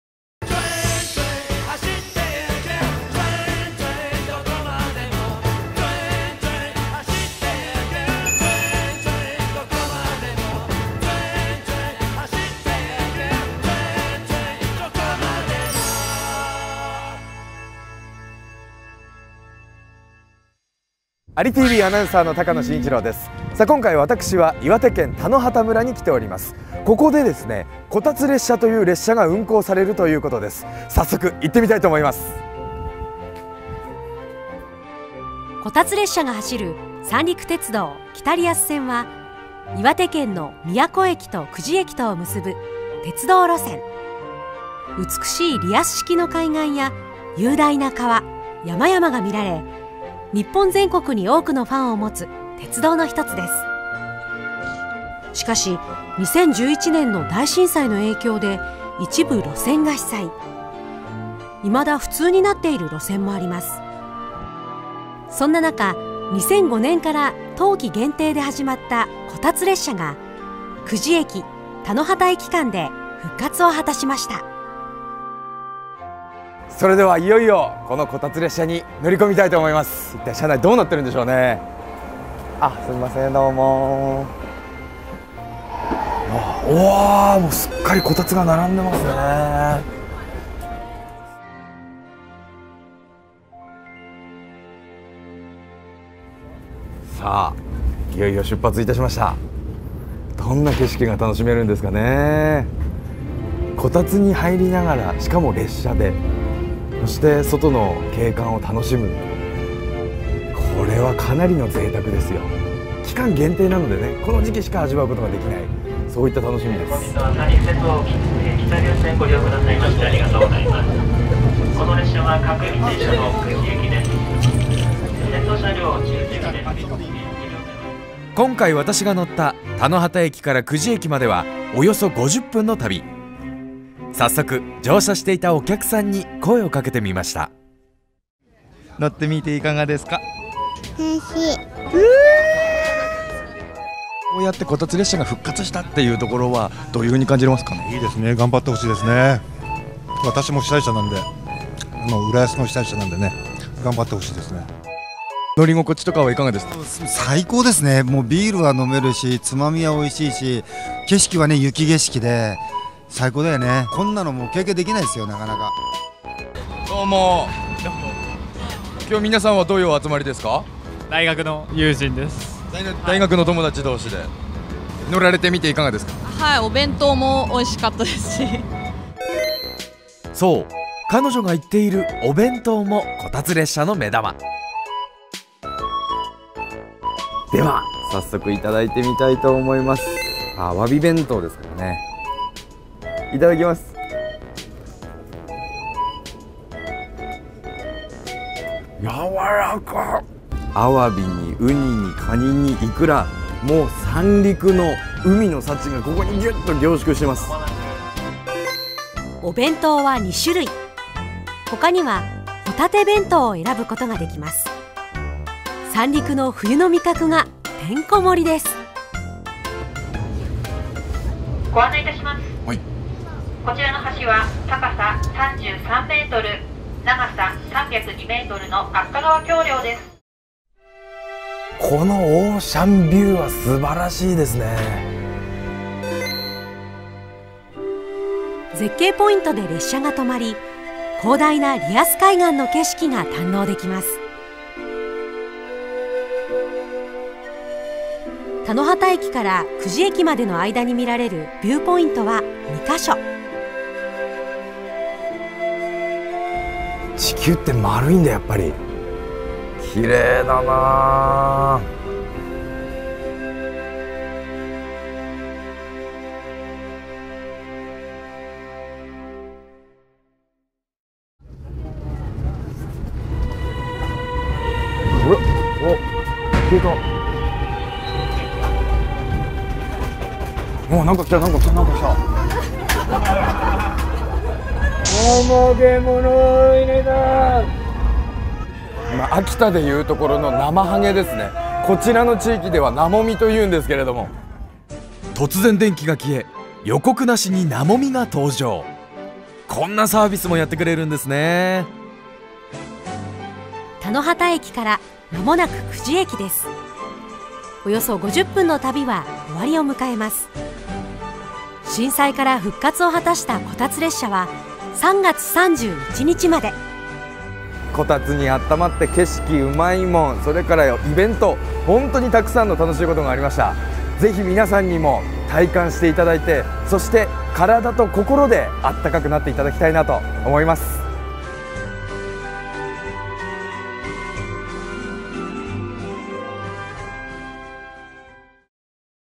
Kotatsu car back after tsunami
Running along the Iwate north coast is the Sanriku Railway's Kita-Rias Line.
Reconstruction after the tsunami remains unfinished, and many parts remain closed.
A popular winter tradition on this line, the kotatsu car, has returned!
(Kotatsu are low heated tables ubiquitous throughout Japan)
Join ariTV's Shinichirō Takano on a sightseeing trip up the spectacular Iwate coast from Tanohata to Kuji.

The kotatsu car runs from mid-December to the end of March, weekends and holidays.